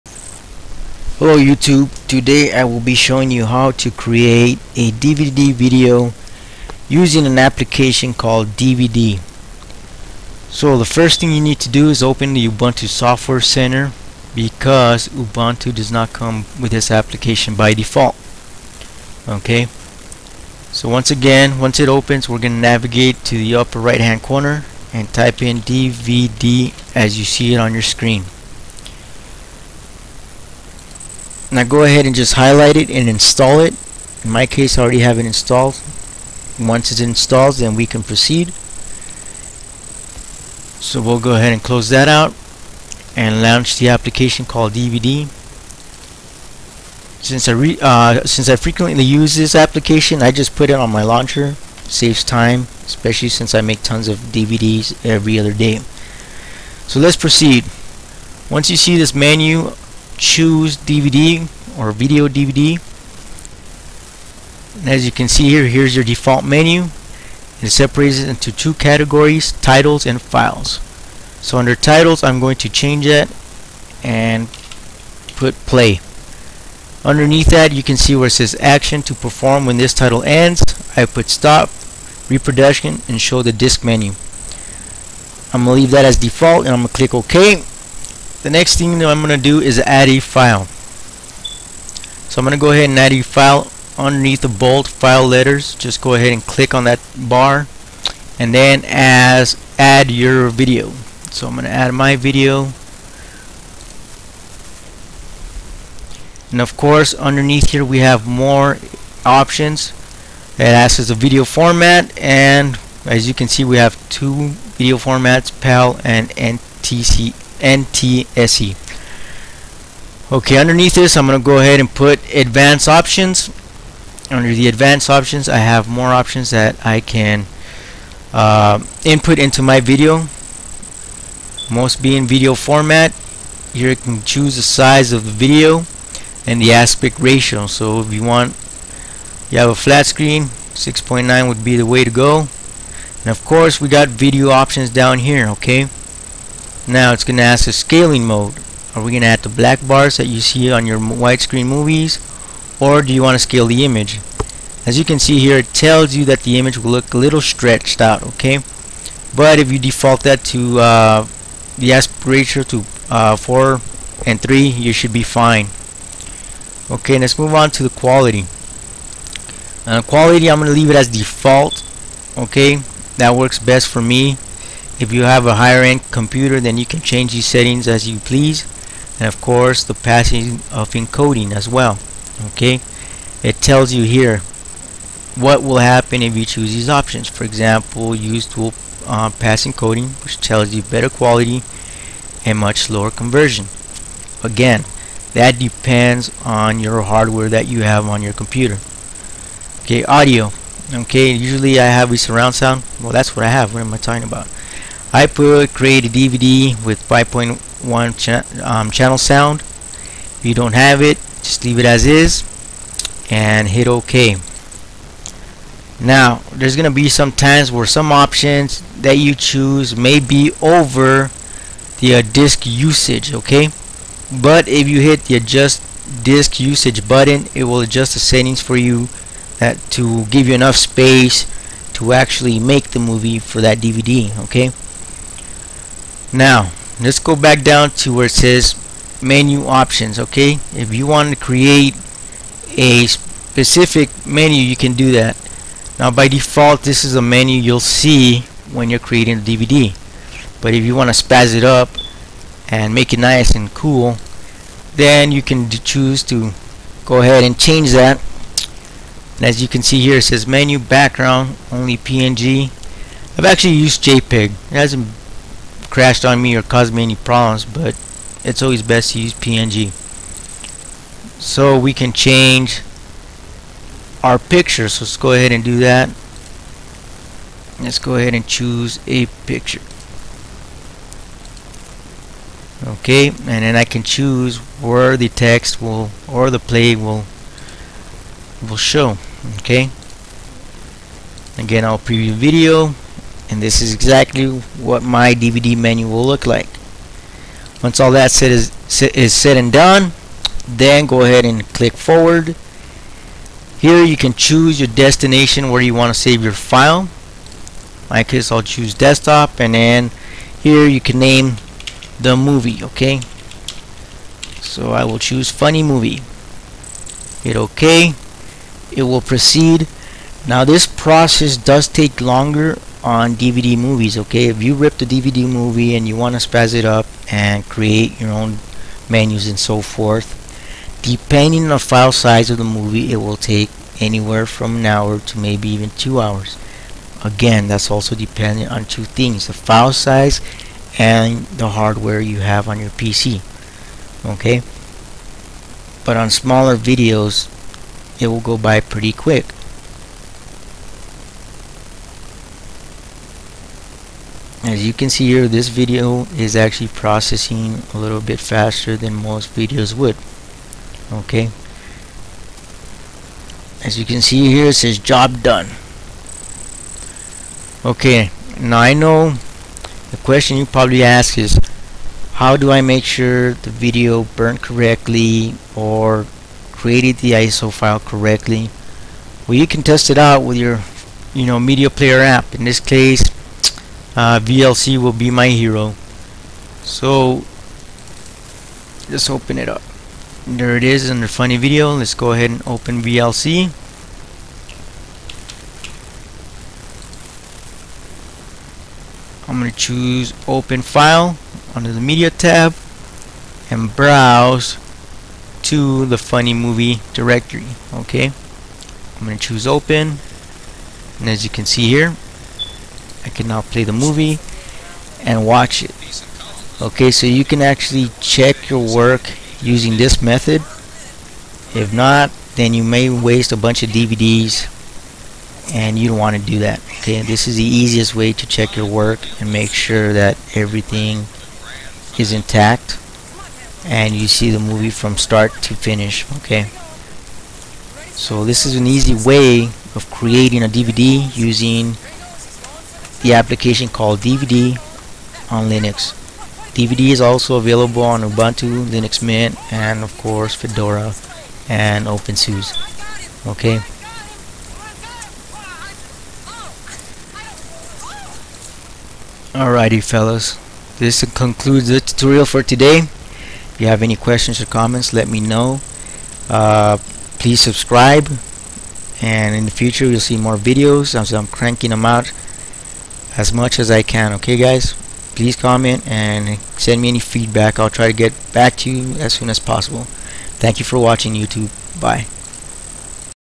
Create DVD Movies on Linux
This video demonstrates how to create DVD movies on Linux.